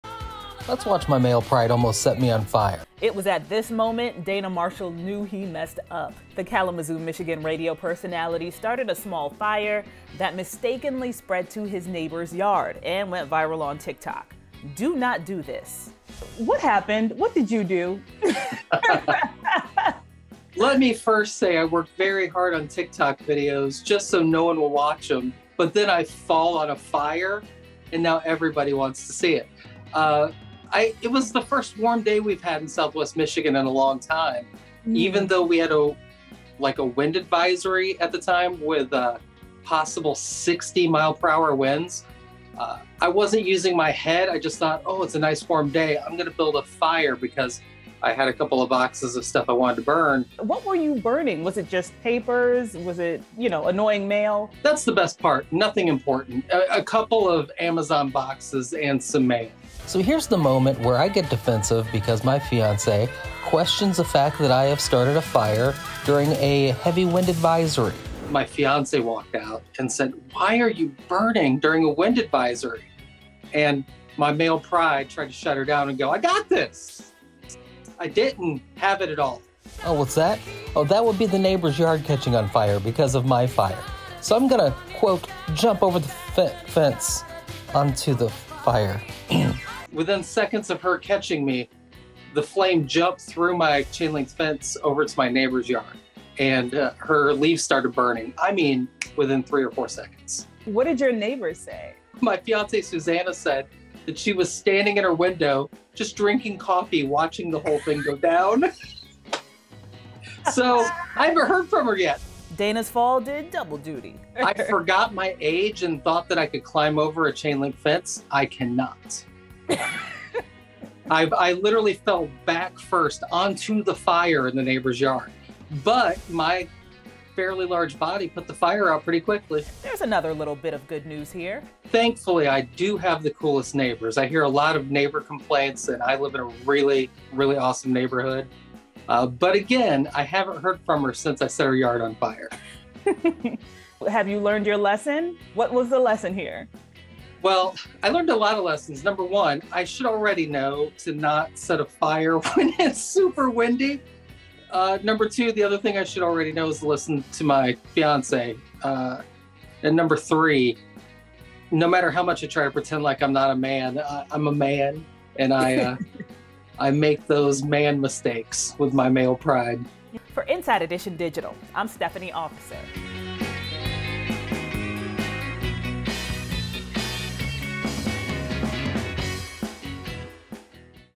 This Man Accidentally Set His Neighbor’s Yard on Fire
All Dana Marshall wanted to do was enjoy Michigan’s first 60-degree day in a long time. Sick of the winter cold and looking for outdoor activities, he decided to do something he has done before: a very small controlled burn. What Marshall got in return is a little more than he bargained for, once the fire spread through his chain link fence. Inside Edition Digital’s Stephanie Officer has the details.